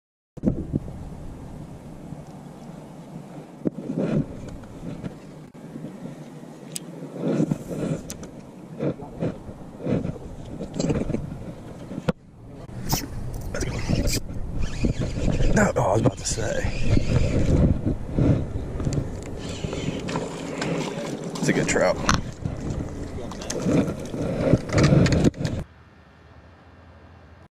Spotted Sea trout Fishing: My Go-To Shrimp Rig for Landing Studs!
My favorite way to rig shrimp on a jig head that always produces fish.